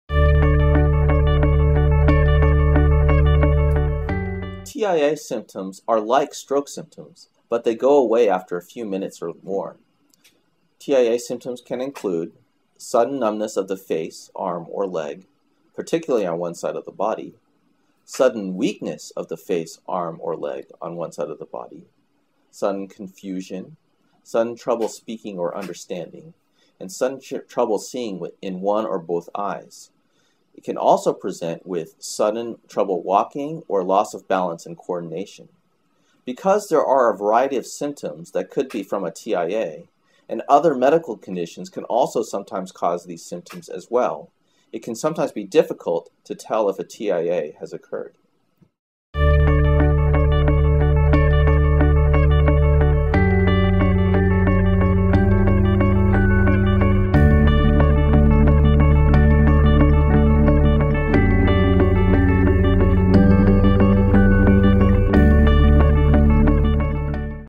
Transient Ischemic Attack (TIA) or Mini-Stroke Symptoms
Anthony S. Kim, MD, a neurologist at the University of California San Francisco, discusses typical symptoms of a transient ischemic attack (TIA) or mini-stroke. For more information about TIA and to learn more about our online TIA research study please visit the WebTIA Project